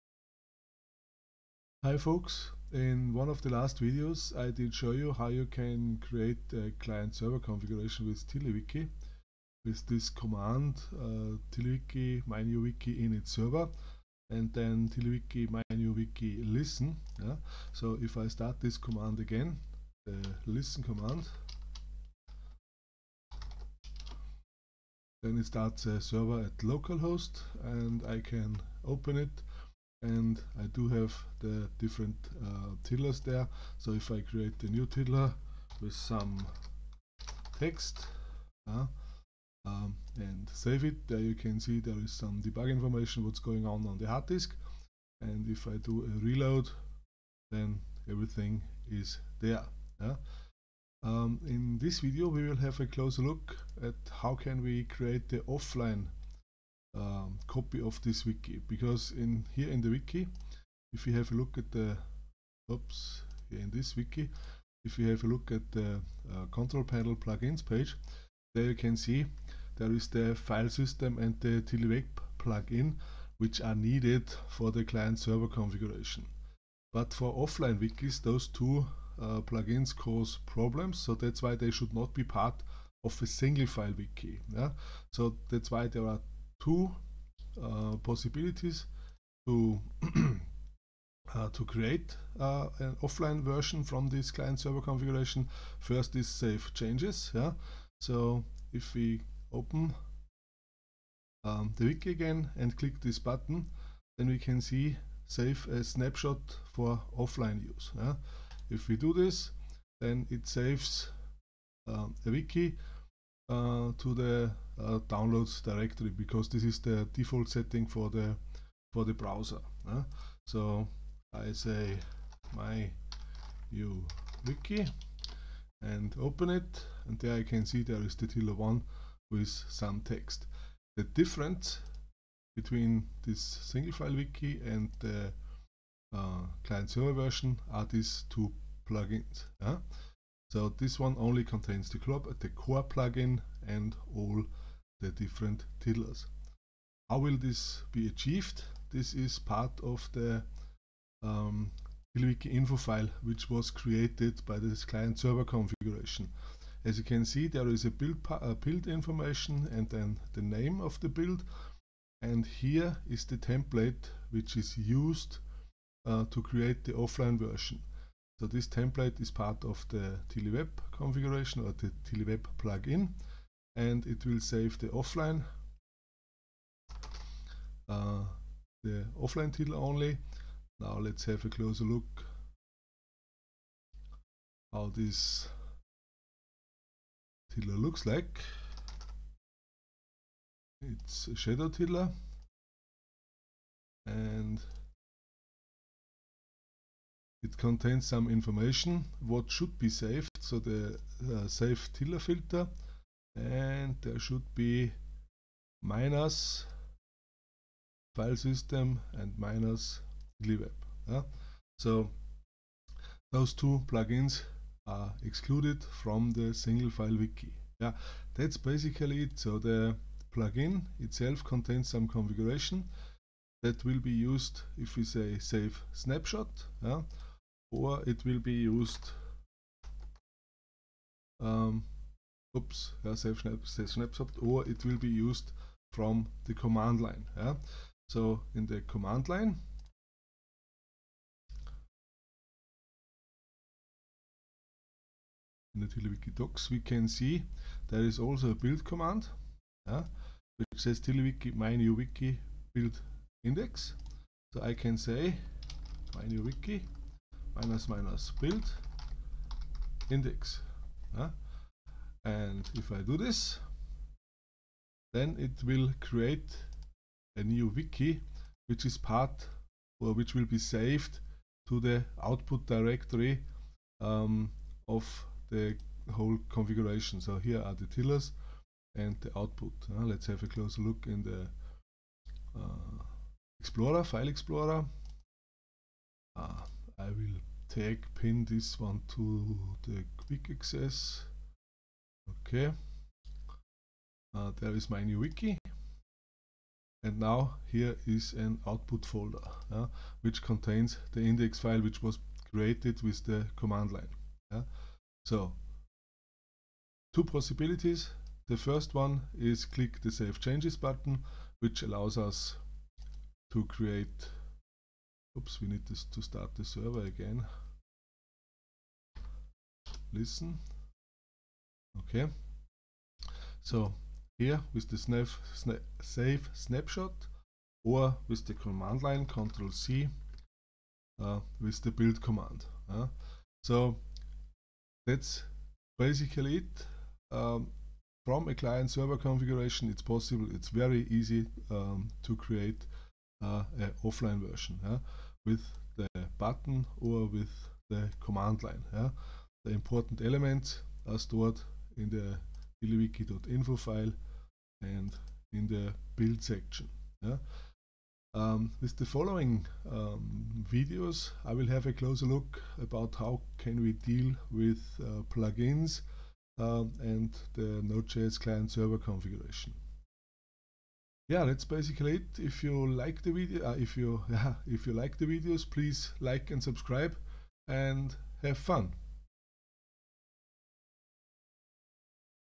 08 Build an Index wiki with the CLI
A short introduction, how to build a tiddlywiki index.html single file wiki using the CLI (command line interface)